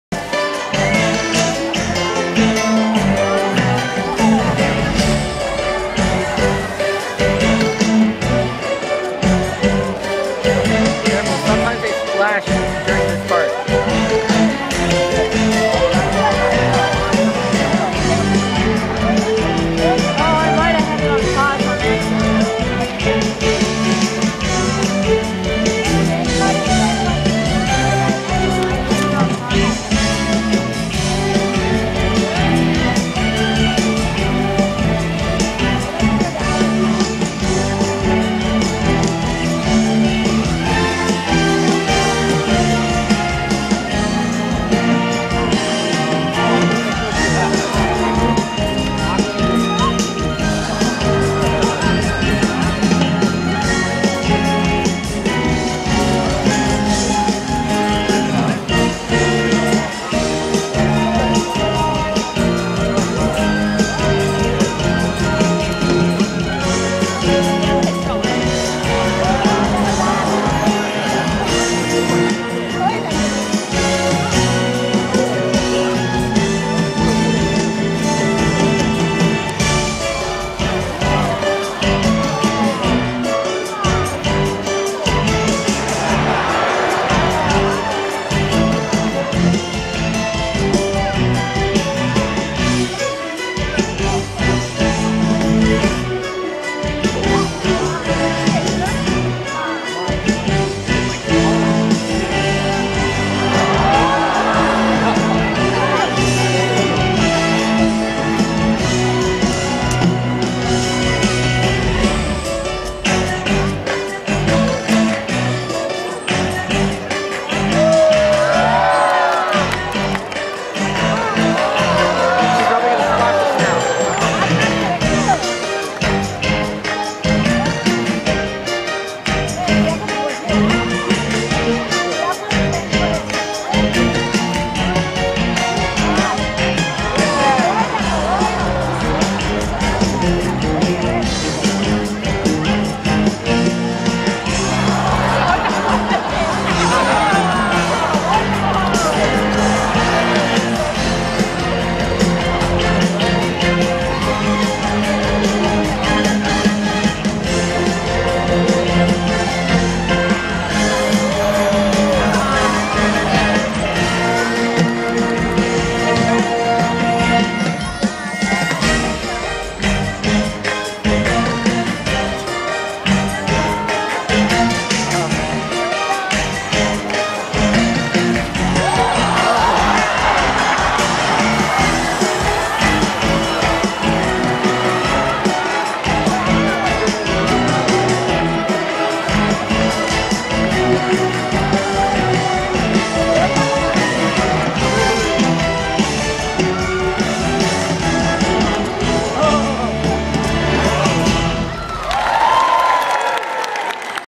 One Ocean part 2
I am uploading these slowly. Corky and Kasatka. 12/27/13.  BLACKFISH RELATED OR ANTI CAPTIVITY COMMENTS WILL BE REMOVED AND YOU WILL BE BLOCKED.